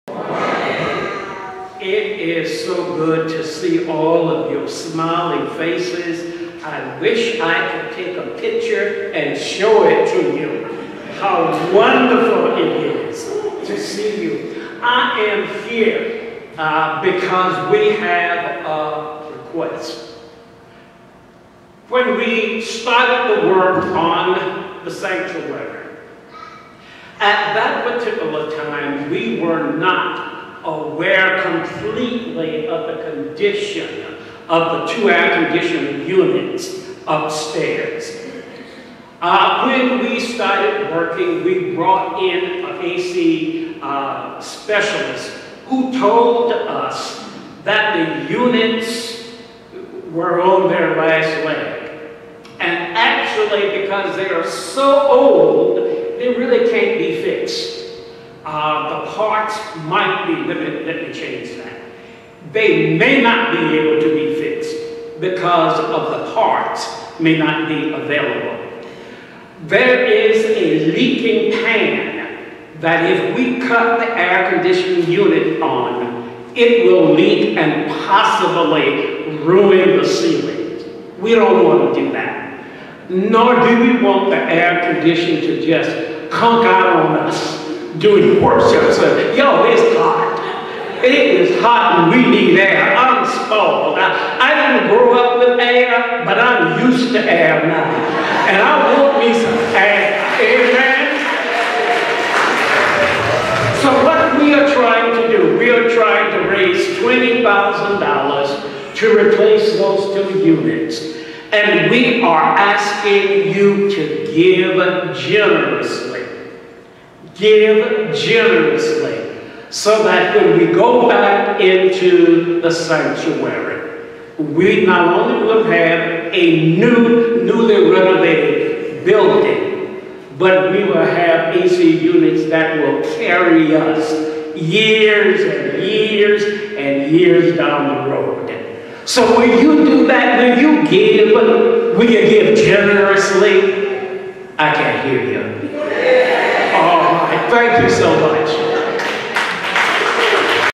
Wesley United Methodist Church - Columbia, SC - 08/02/ 2018 - Condition of the Air condition
Wesley  United Methodist Church - Columbia, SC - 08/02/ 2018 - Condition of the Air condition at Wesley